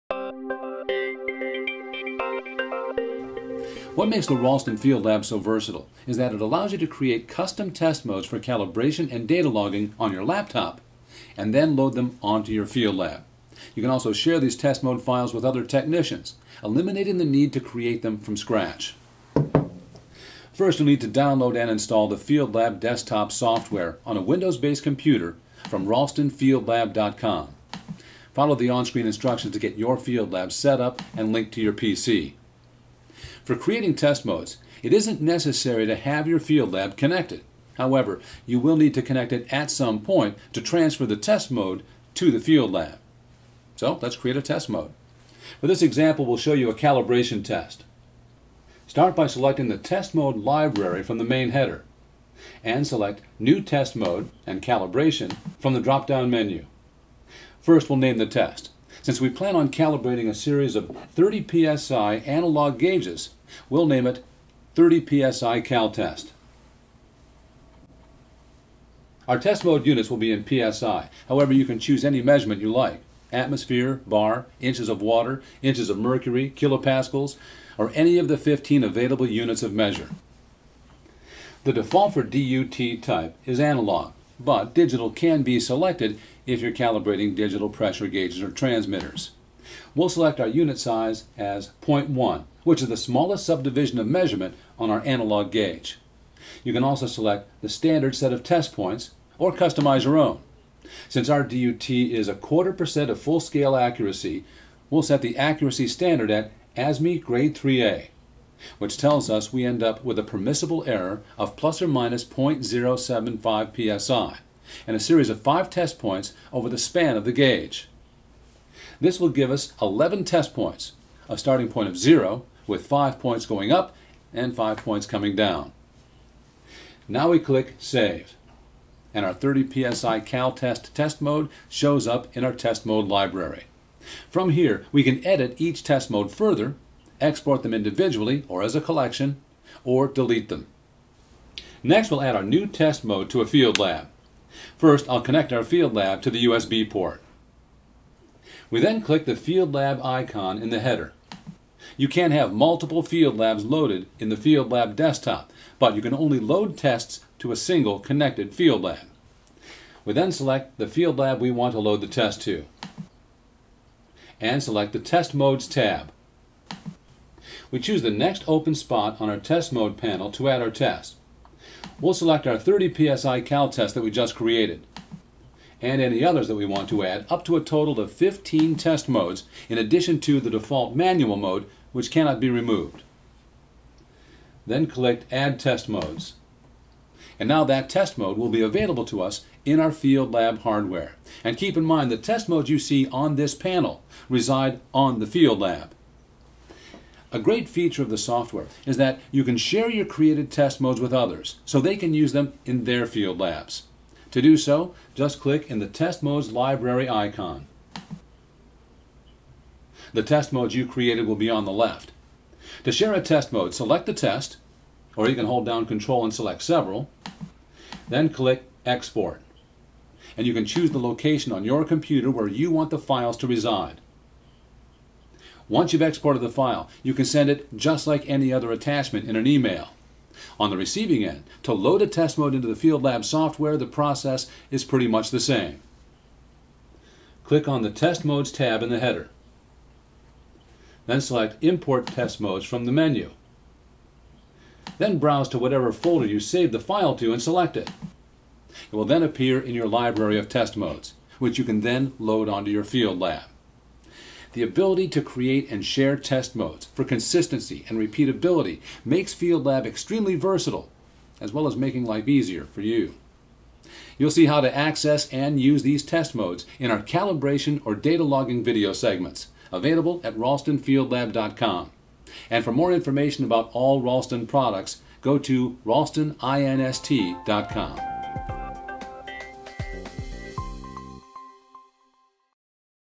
How to Create and Share Calibration Test Modes with Digital Pressure Calibrator
Creating and Sharing Calibration Test Modes - We can make calibration test modes for each type of digital pressure device easily by adding to one or multiple FieldLabs or share with others. Test Point adapters are used for time-saving and secure connections to any device.